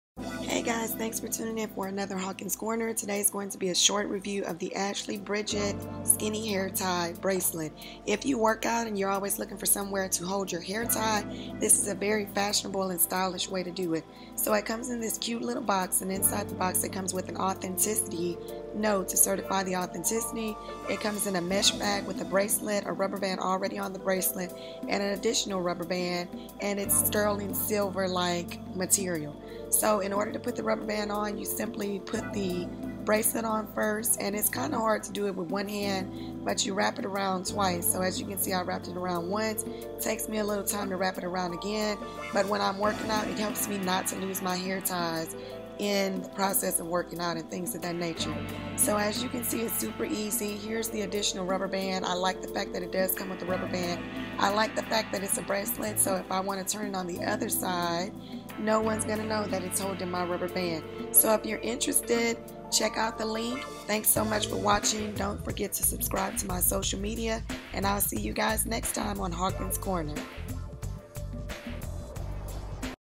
Ashley Bridget Skinny Hair Time Bracelet
Check out this fashionable way to hold on to your hair tie! 😘

Product Information:
Company:  Ashley Bridget
Product: Skinny Hair Tie Bracelet
Price: $11.85
Where to Purchase